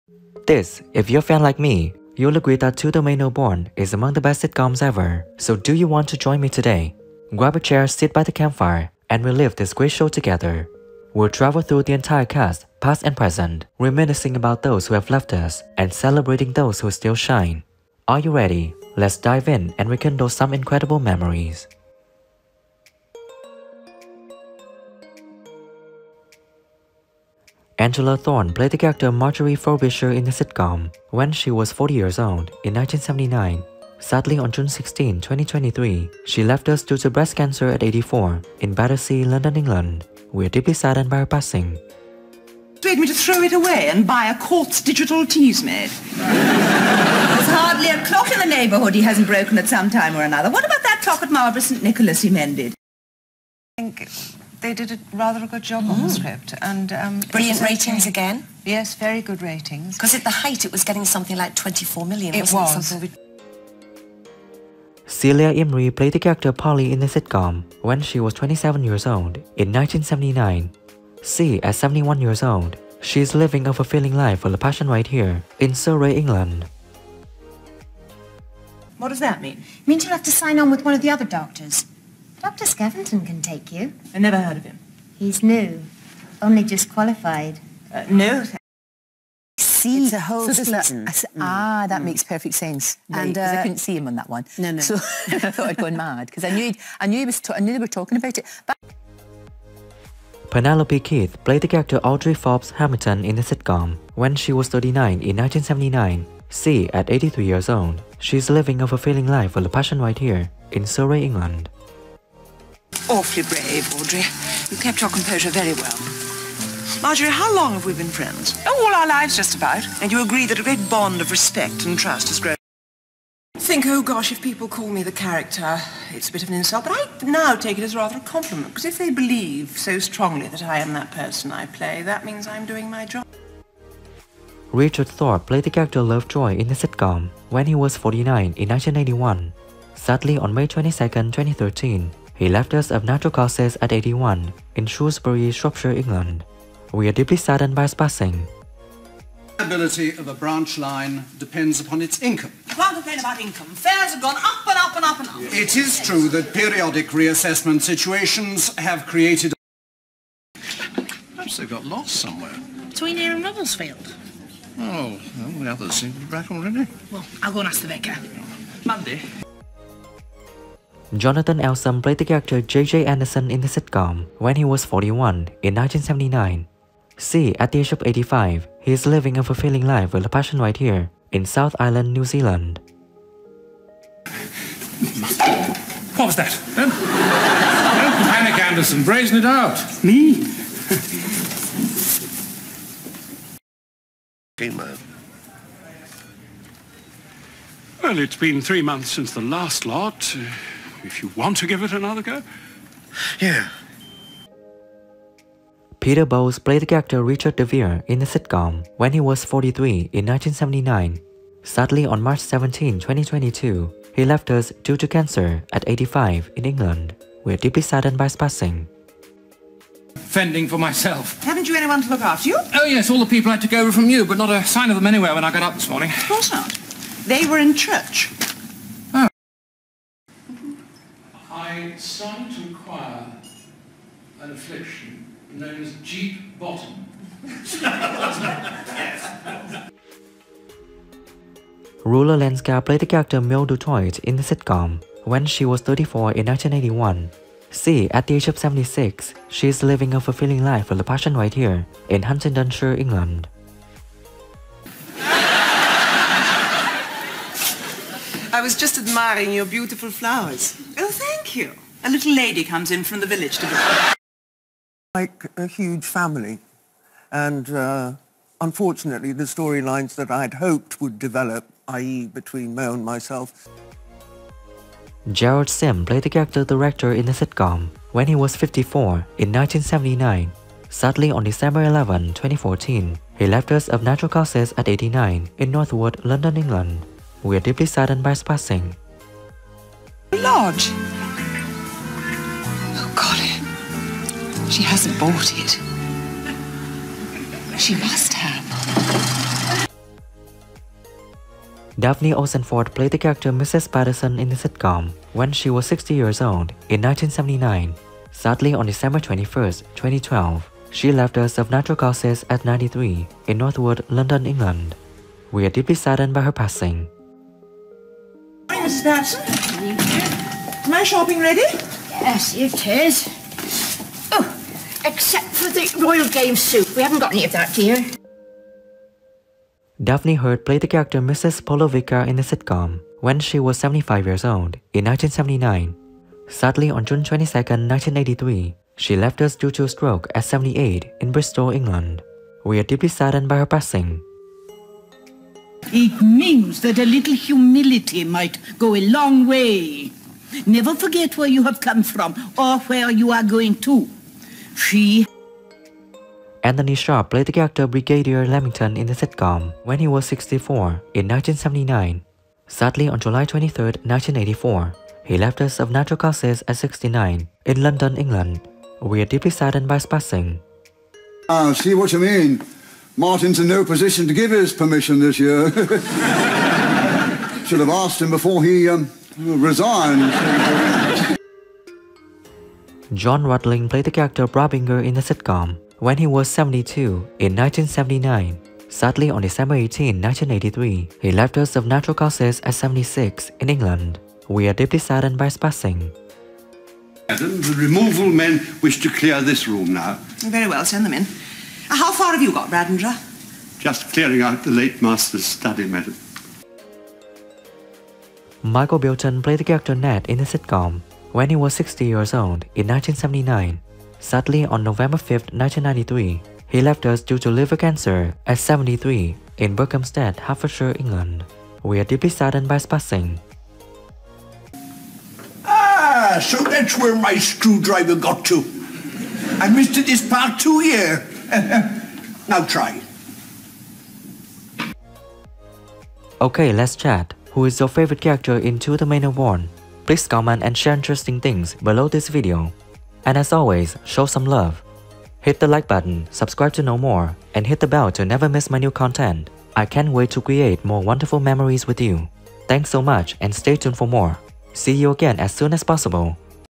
TO THE MANOR BORN 1979 Cast THEN AND NOW 2024 All Actors Have Aged Terribly 😢
To the Manor Born is a BBC television sitcom that first aired on BBC1 from 1979 to 1981. A special one-off episode was produced in 2007. Starring Penelope Keith and Peter Bowles, the first 20 episodes and the 2007 special were written by Peter Spence, the creator, while the final episode in 1981 was written by script associate Christopher Bond. The title is a play on the phrase "to the manner born," from Shakespeare's Hamlet ("Though I am a native here and to the manner born, it is a custom more honoured in the breach than the observance".)